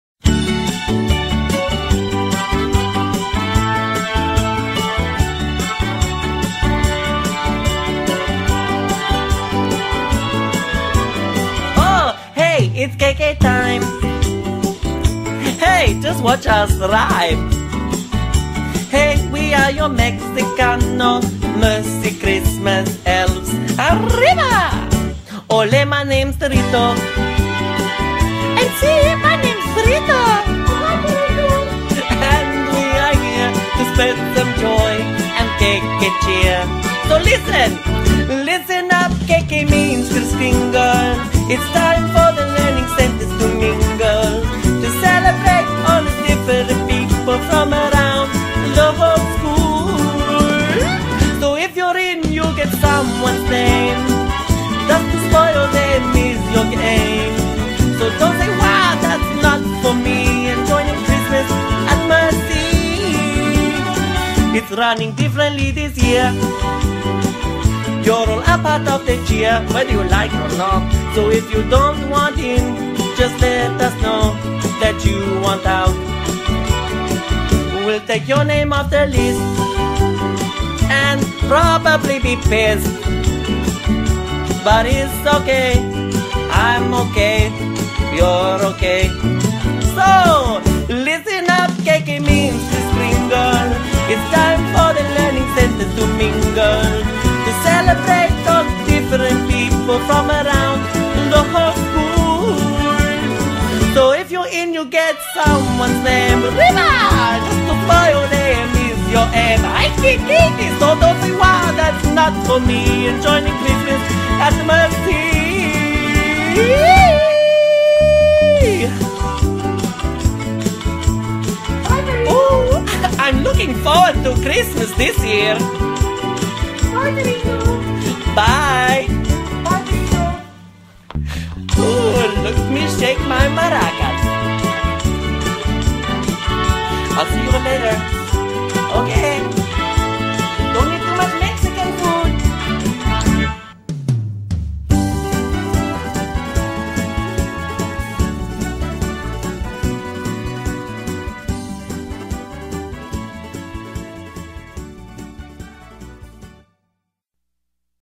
KK 2014 INTRODUCTION VIDEO
Mercy College Staff 2014 - Introduction to Kris Kringle featuring the Mercy Christmas Elves